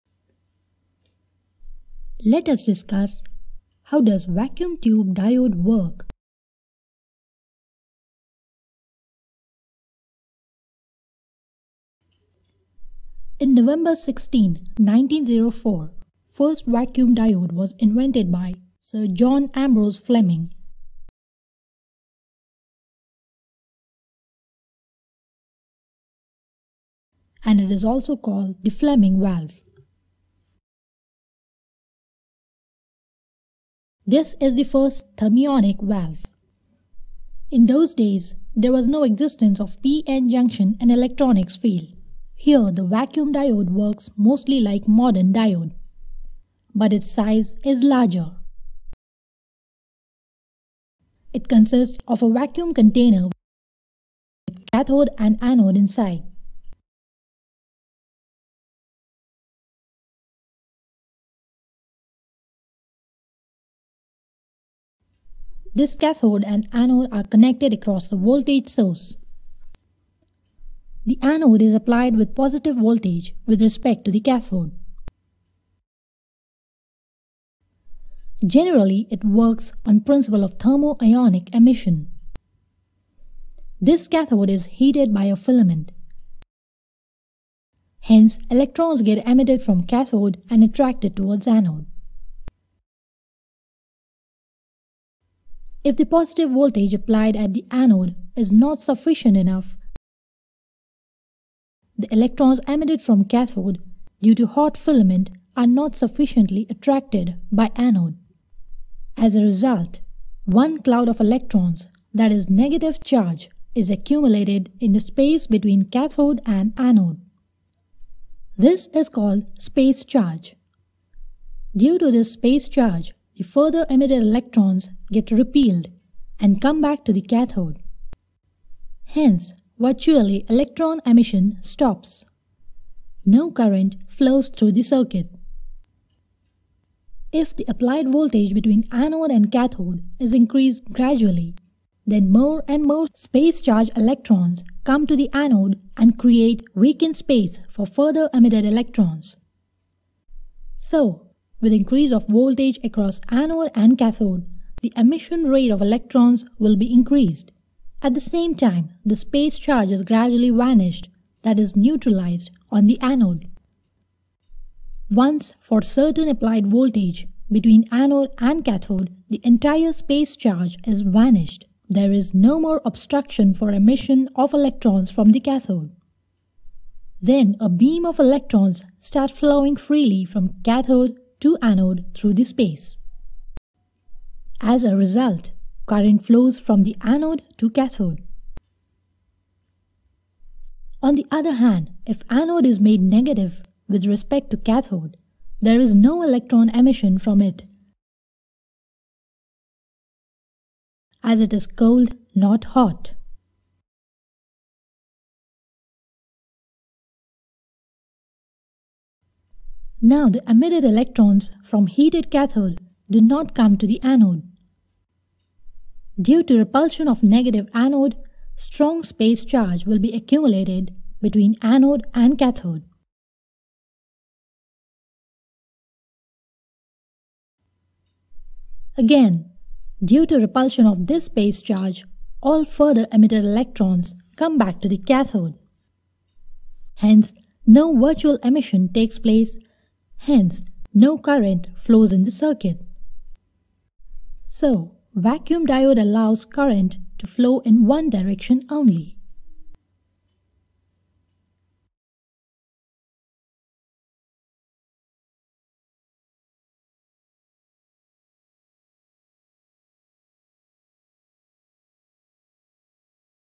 Vacuum Diode
We have explained here basic theory of Vacuum Diode and working principle of Vacuum Diode